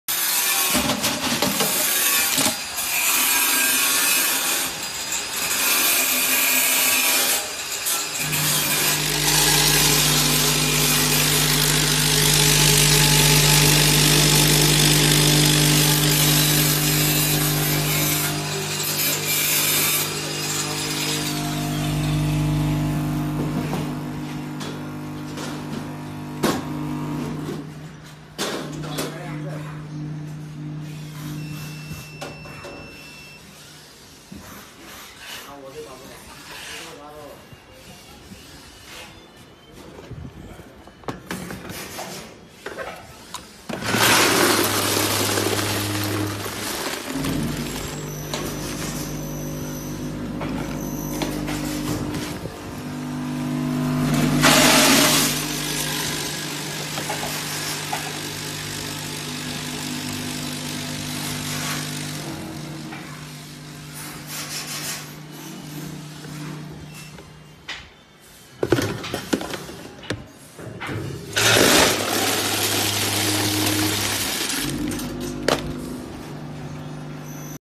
FF2500 Peanut Filling Machine
The powder weigh filling machine works for the peanut, dry nuts, bean etc. from 5g to 2500g base on the products. It can weigh and fill the products into the preformed bags.